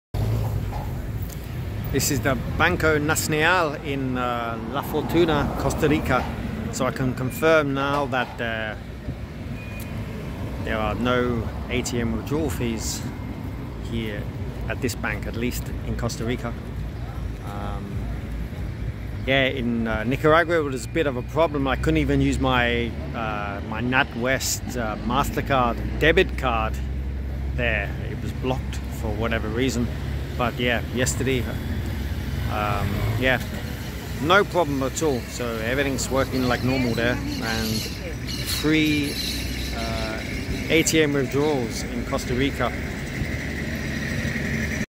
ATMs in Costa Rica - Free Withdrawals at the Banco Nacional
I confirm: No withdrawal fees when using a UK highstreet bank debit card issued by Mastercard at the Banco Nacional in Costa Rica

Probably true of the other banks here too.